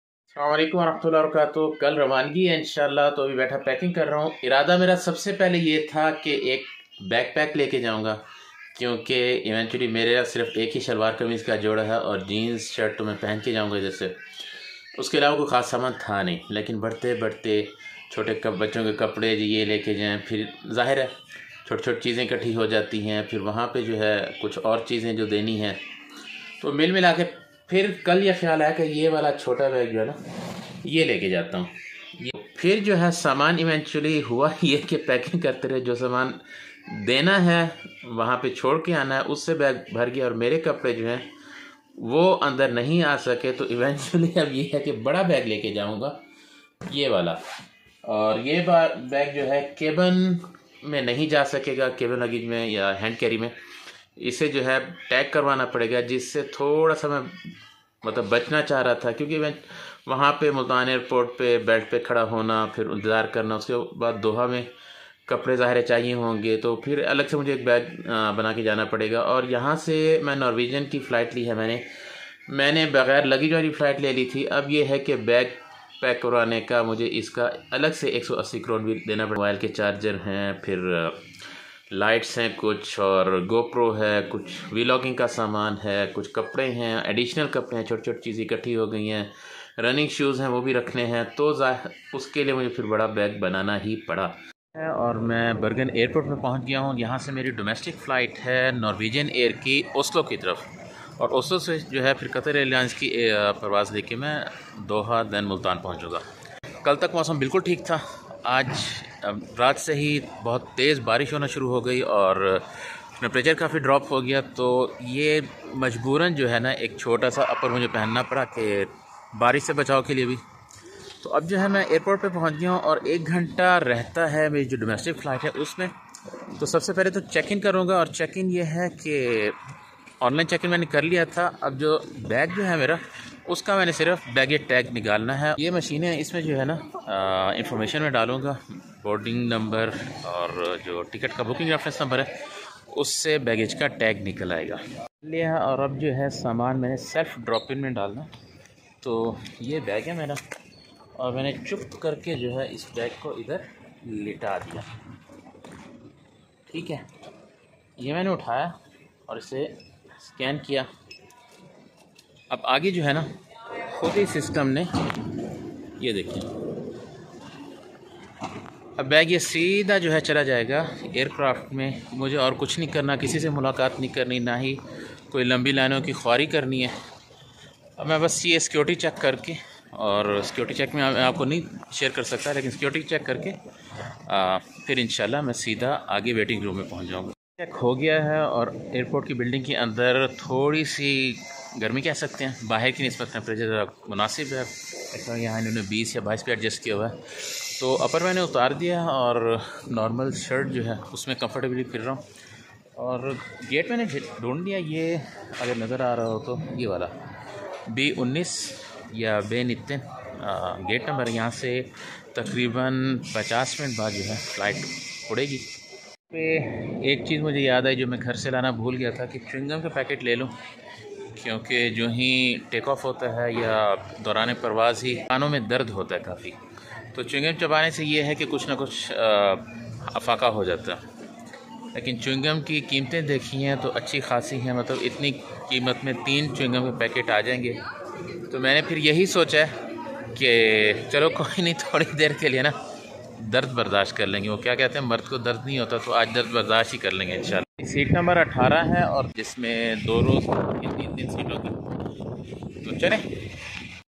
Packing and pehli flight bergen norway vlog 14
That's how I took my domestic flight